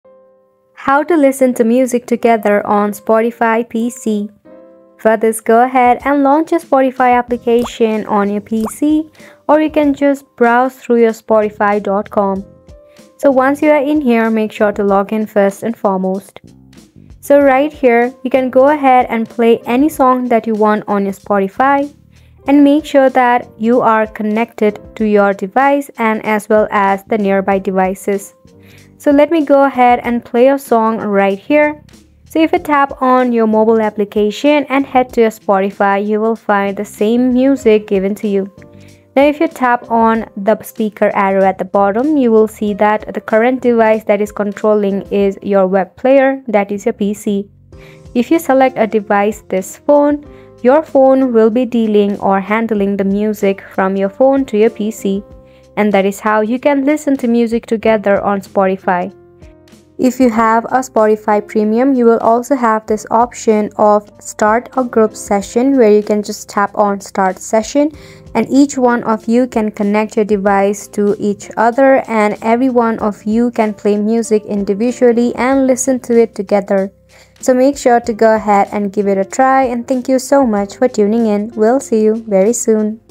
How To Listen to Music Together on Spotify PC Only WAY!
Hey everyone, in this video, I'll show you how to listen to music together on Spotify PC. If you've ever wondered, "How do I enjoy Spotify with friends on my computer?" you're in the right place! Join me as I walk you through the process step by step, making it easy for you and your friends to enjoy your favorite tunes together on Spotify's desktop app.

Timestamps:

0:00 - Introduction: Exploring Spotify's Group Listening Feature
0:10 - How do I Set Up Group Listening on Spotify PC?
0:20 - Step-by-Step: Enjoying Music Together with Friends